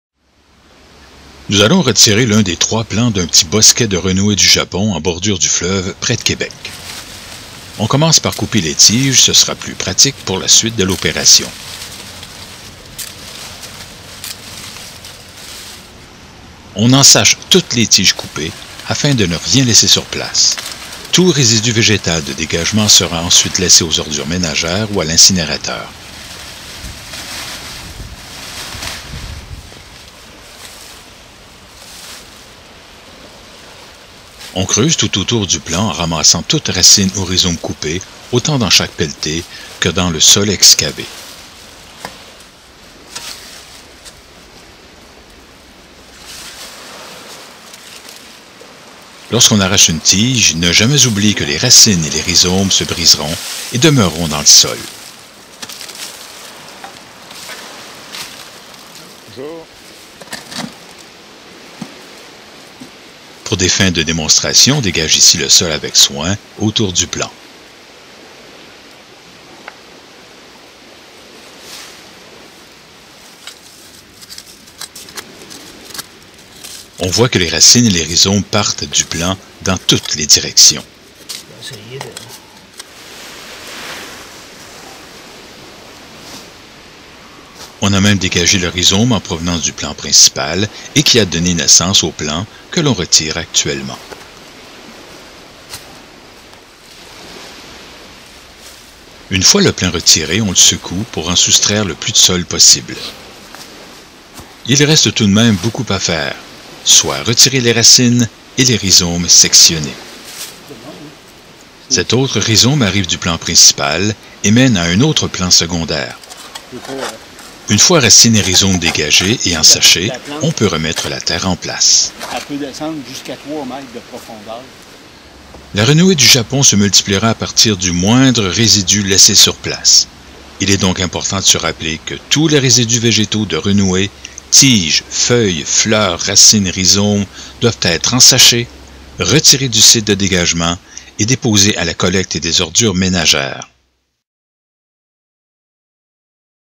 Renouée du Japon - Arrachage
Démonstration d'arrachage d'un plan de renouée du Japon. Technique usuelle et précautions.